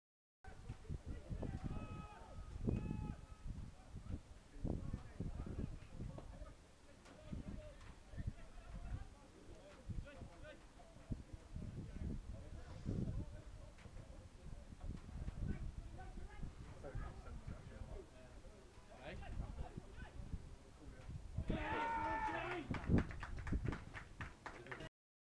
Duchy Div3 Game, St Stephen Res 0 v 5 Polpero Res
Aiden Le Masurier is given a yellow card for this challenge.